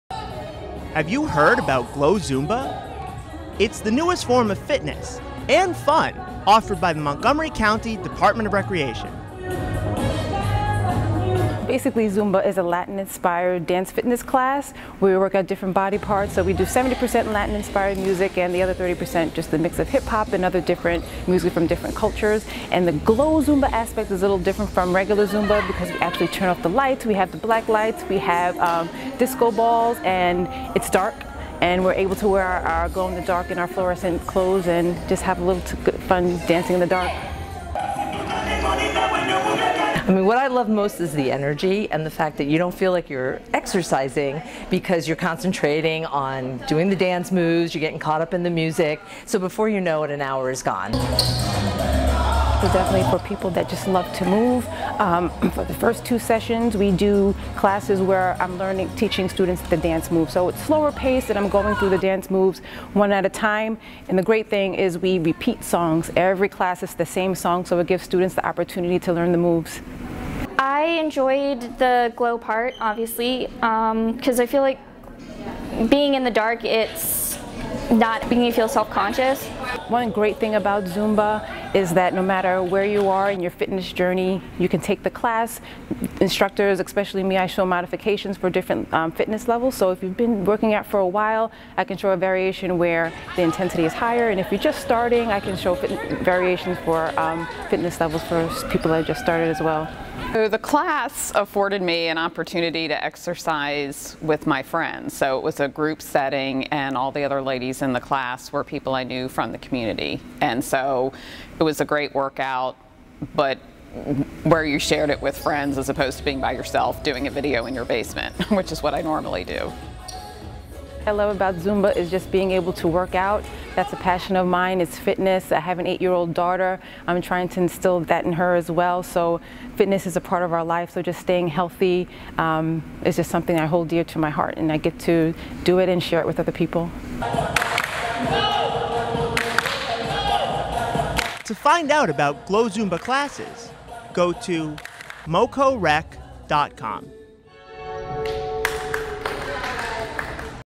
Glow Zumba at Montgomery County Recreation
The Montgomery County Dept. of Recreation is now offering Glow Zumba classes. Check MoCoRec.com for details.